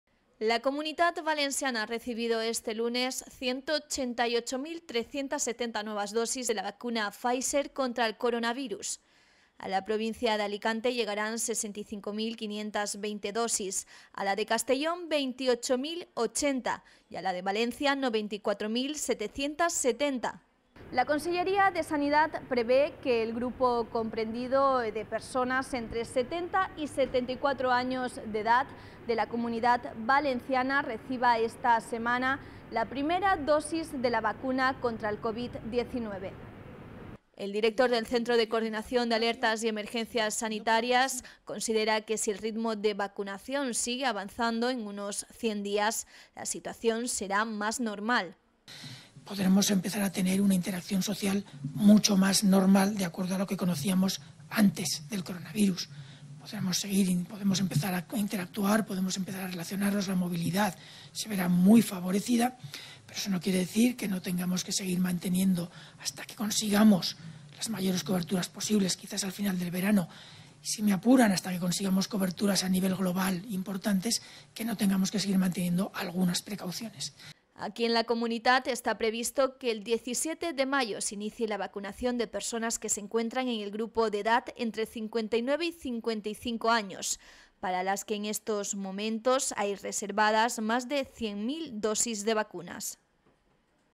Llegan a la Comunitat Valenciana 188.370 nuevas dosis de la vacuna de Pfizer
La Conselleria de Sanidad prevé que todas las personas de la Comunitat Valenciana del grupo de edad comprendido entre los 70 y los 74 años habrán recibido esta semana al menos una dosis de la vacuna frente a la covid-19.